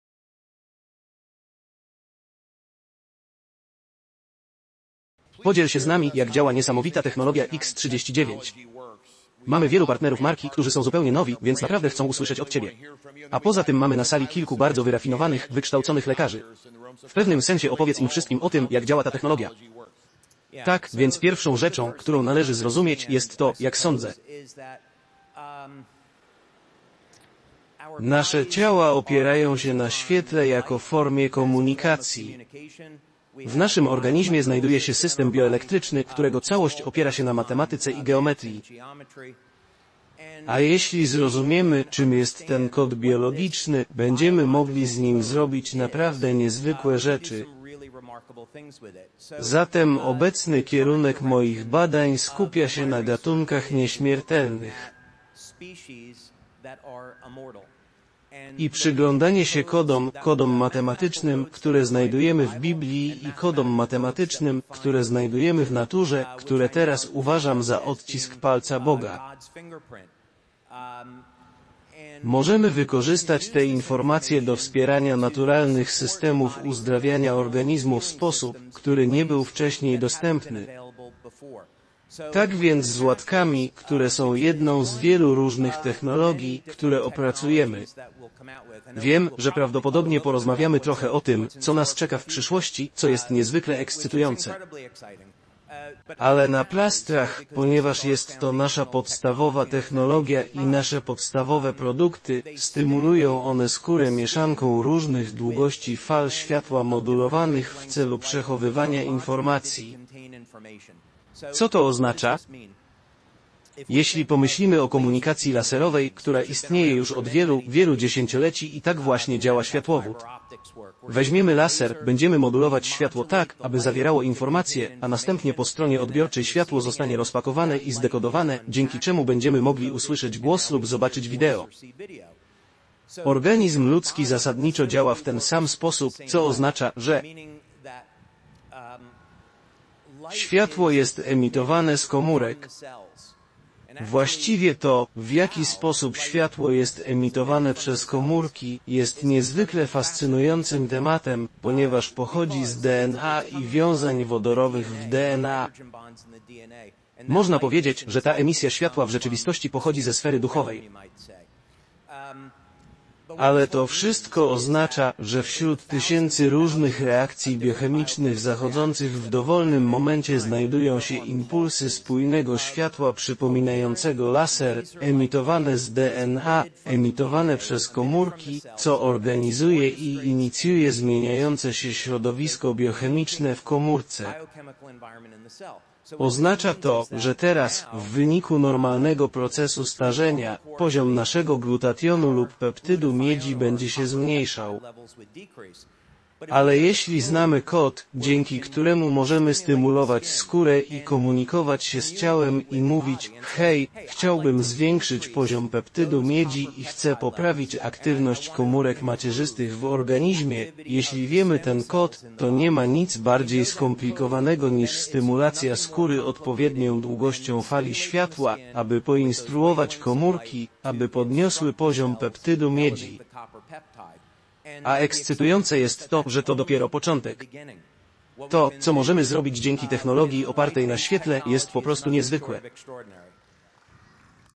Odcisk Palca Bożego w Uzdrawianiu: Technologia X39 - naturoterapia.pl
Film dotyczy technologii X39, która opiera się na odkryciu, że ludzkie ciało wykorzystuje światło jako formę komunikacji. W organizmie znajduje się bioelektryczny system oparty na matematyce i geometrii, czyli swoisty "kod biologiczny". Zrozumienie tego kodu pozwala na dokonywanie niezwykłych rzeczy.

Prelegent wspomina, że jego badania koncentrują się na gatunkach nieśmiertelnych oraz na kodach matematycznych występujących w Biblii i w naturze, które określa "odciskiem palca Boga". Te informacje można wykorzystać do wspierania naturalnych systemów uzdrawiania organizmu w sposób dotychczas niedostępny.

Jedną z technologii opracowanych w oparciu o te odkrycia są plastry, które stymulują skórę mieszanką różnych długości fal światła modulowanych w celu przechowywania informacji. Działa to na zasadzie komunikacji laserowej - światło jest modulowane, aby zawierało informacje, a następnie jest ono rozpakowane i zdekodowane po stronie odbiorczej.

Organizm ludzki funkcjonuje w analogiczny sposób - komórki emitują spójne impulsy światła, które organizują i inicjują zmiany w środowisku biochemicznym komórki. Wraz z wiekiem następuje obniżenie poziomu niektórych substancji, takich jak glutation czy peptyd miedzi. Jednak dzięki stymulacji skóry odpowiednią długością fali światła można instruować komórki, aby zwiększyły produkcję tych substancji, co pozwala na poprawę funkcjonowania organizmu.

Prelegent podkreśla, że to dopiero początek tego, co można osiągnąć dzięki technologii opartej na świetle. Możliwości tej technologii są po prostu niezwykłe.